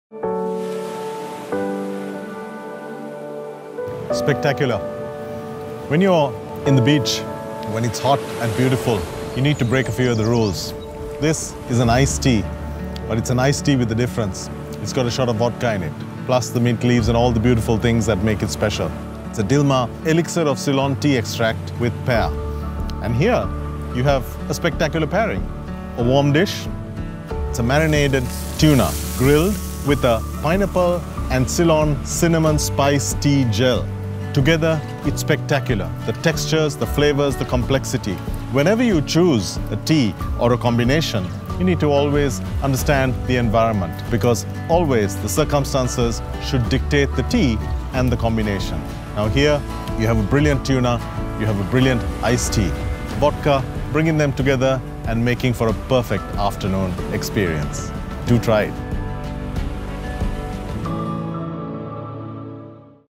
An Afternoon Foodscape at Cape | Vlog
If your teatime still looks like tea and scones, you aren’t doing it right! Our travelling Tea Grower and Dilmah CEO, Dilhan C. Fernando, opened up about travel and tea in his vlog series, Tea Inspired on Location.
Join him on a travel and taste adventure as he explores Sri Lanka and 21st century teatime experiences at Cape Weligama - Relais & Chateaux.